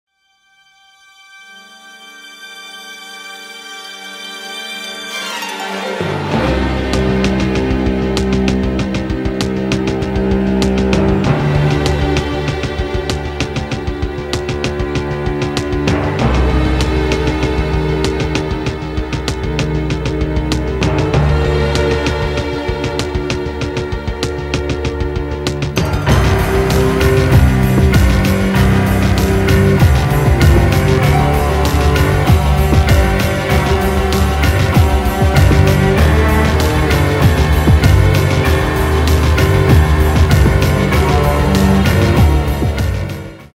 Every Bjork song from least to most played on Spotify (trailer)
The trailer 4 the most waited video I guess again, Every Bjork song from least to most played on Spotify
Launching on the same day the copycat biatch launches her album TOTALLY ORIGINAL